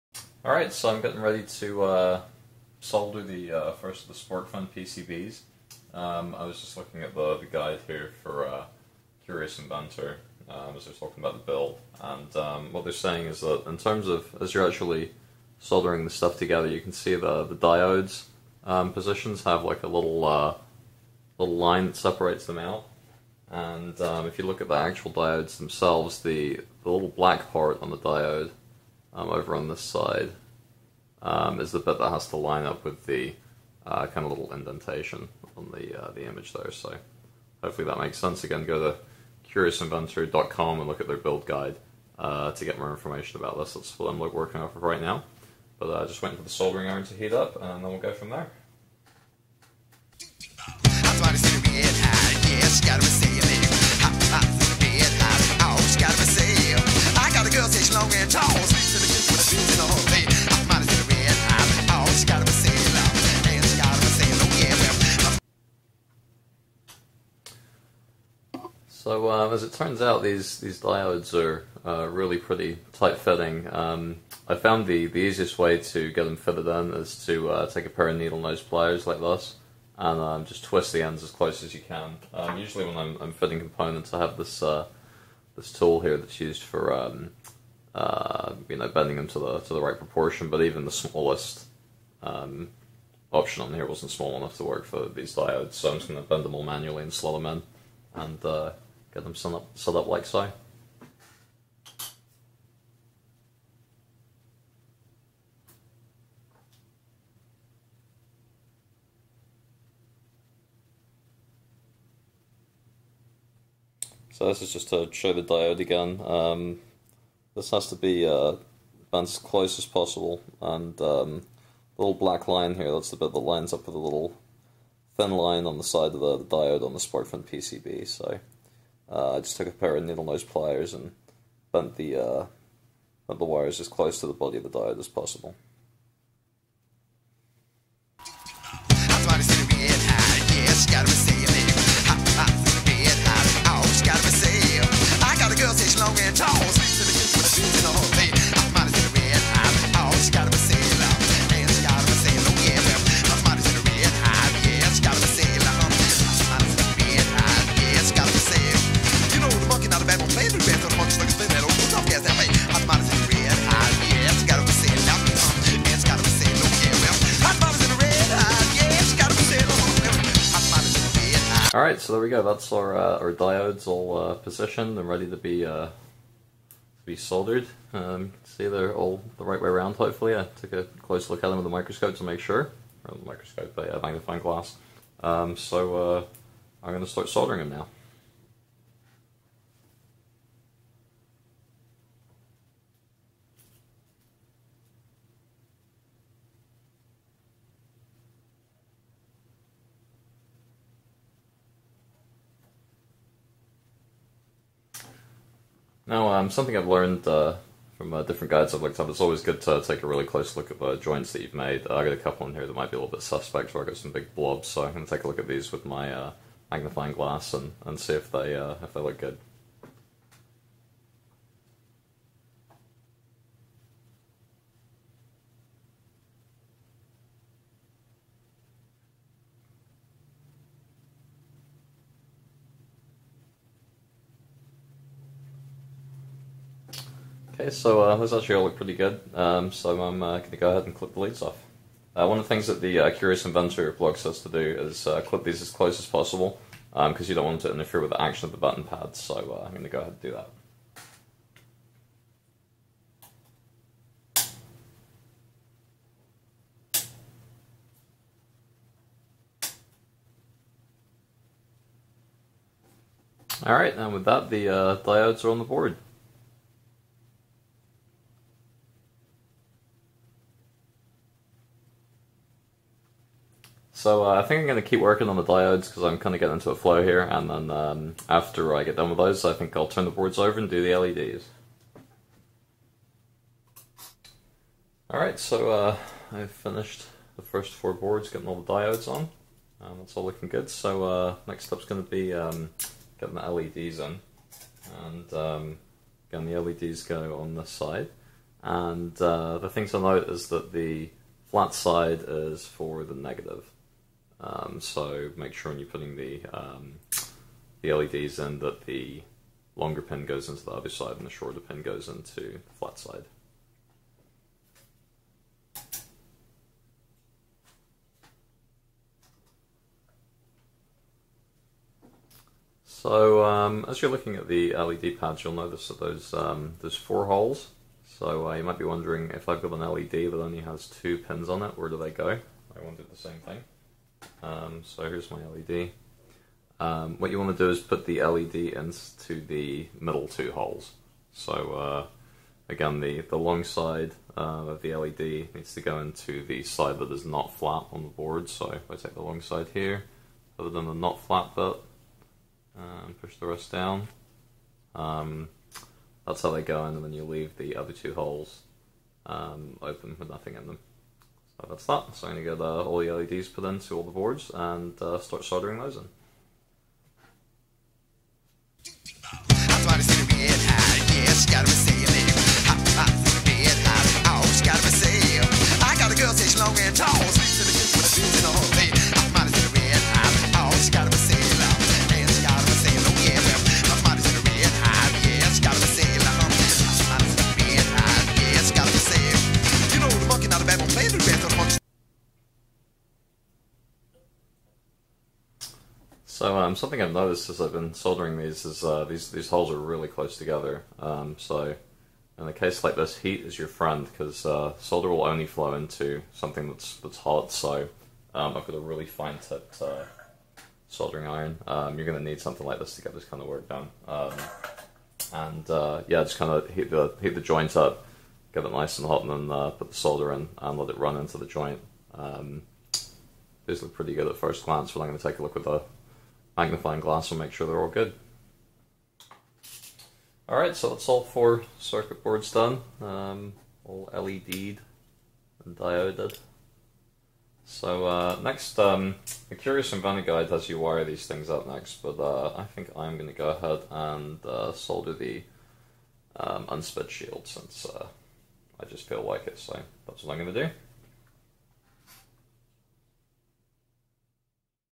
3 - How to Build an Arduinome: Diodes and LEDs
In this part of the series, we look at how to install and solder the diodes and LEDs that make up the face of the Arduinome.